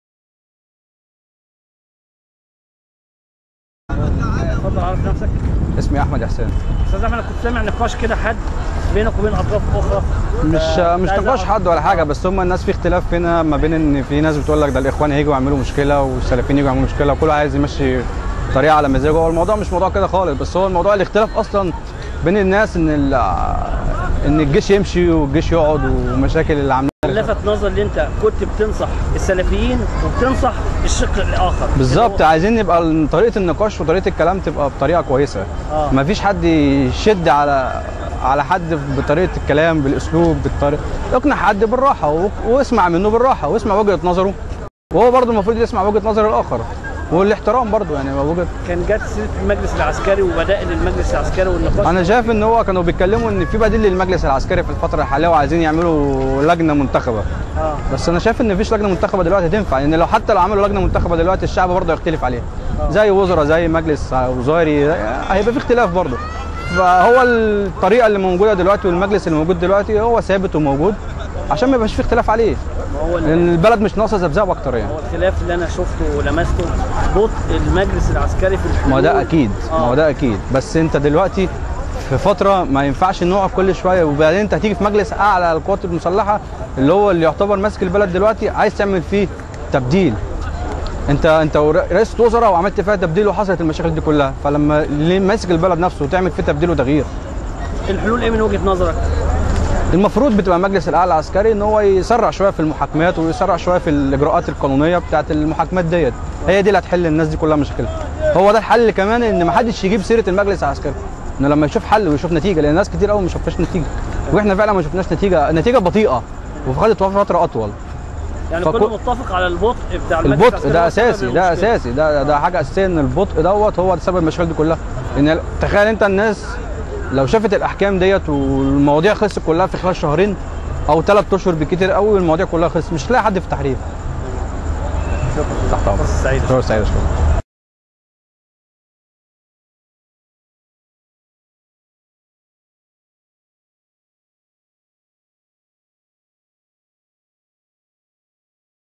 أحد شباب التحرير يوضح الإختلاف فى الميدان
مساء يوم الأربعاء أحد شباب التحرير يوضح الإختلاف فى الميدان